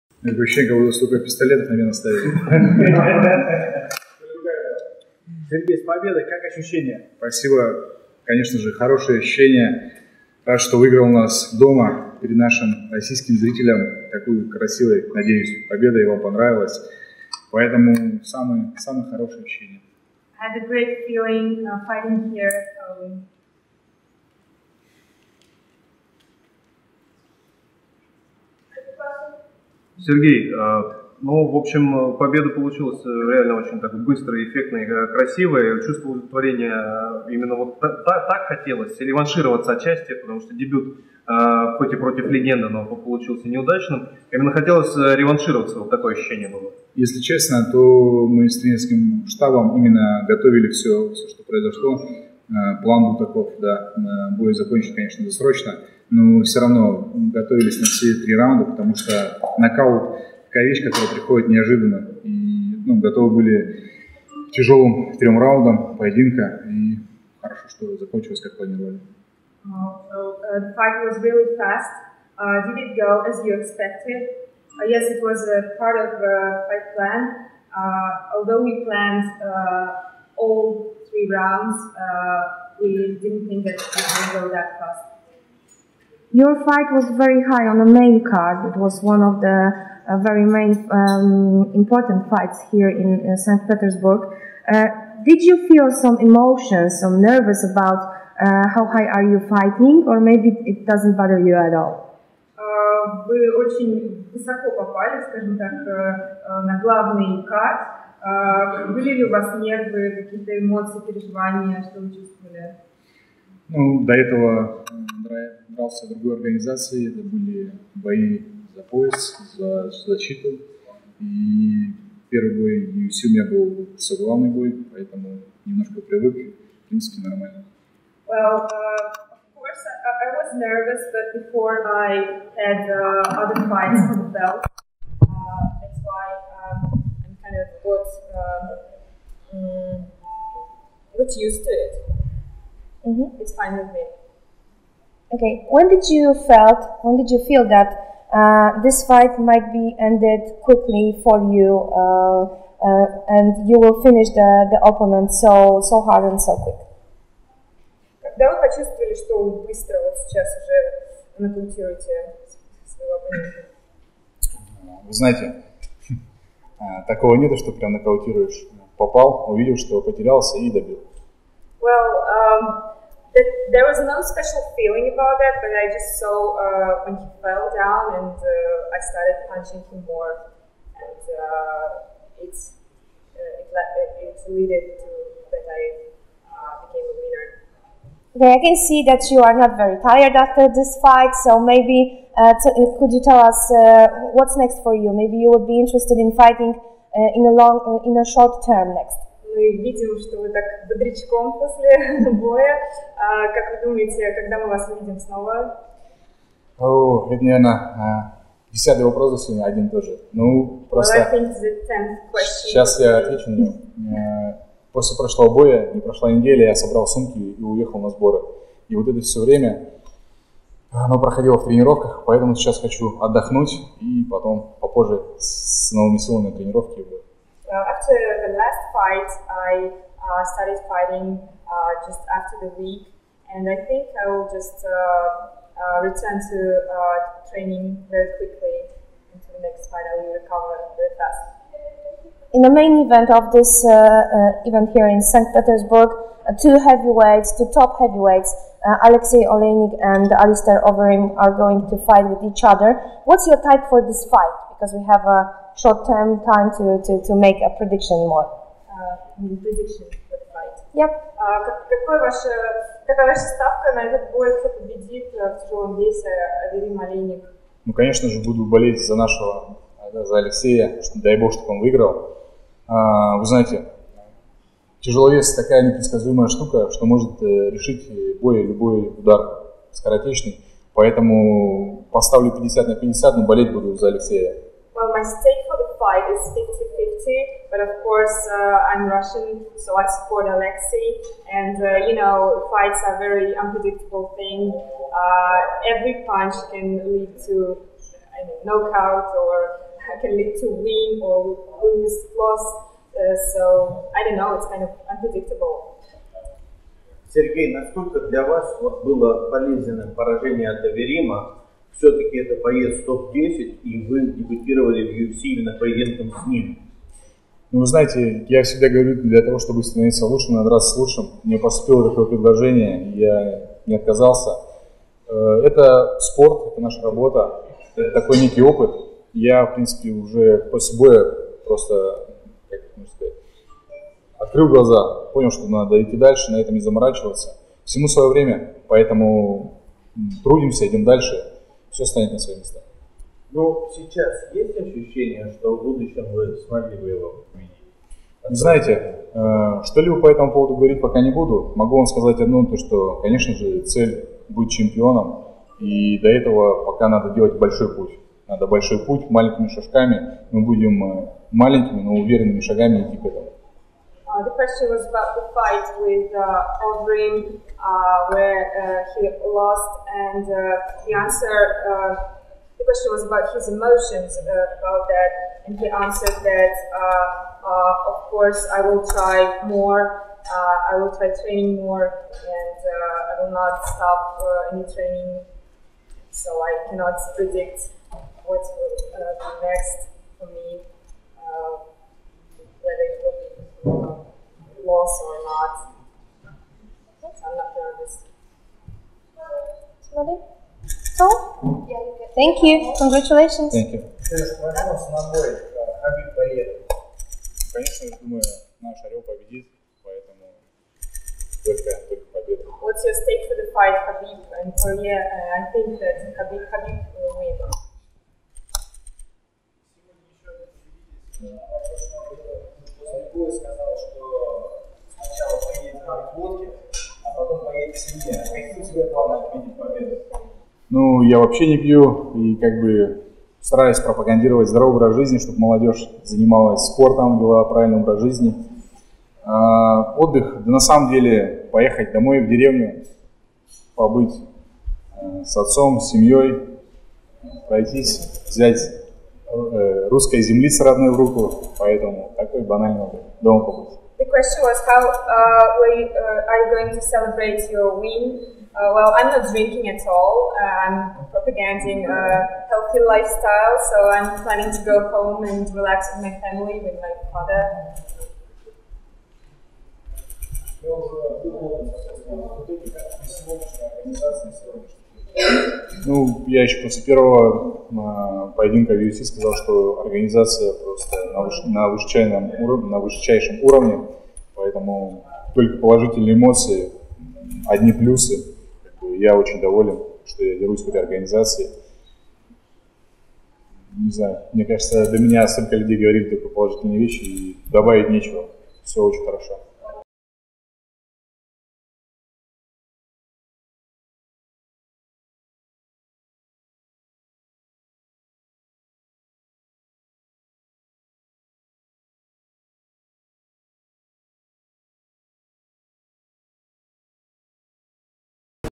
Сергей Павлович: Цель прежняя – быть чемпионом
Записав на свой счет первую победу под баннером UFC, российский тяжеловес Сергей Павлович подвел итоги поединка с бразильцем Марсело Голмом, а также сообщил о том, что ставит перед собой в организации самые большие цели.
Напомним, что рефери Леон Робертс остановил бой тяжеловесов уже на 66-й секунде первого раунда, зафиксировав победу Павловича нокаутом. По итогам мероприятия россиянин стал обладателем турнирного бонуса за «Выступление вечера».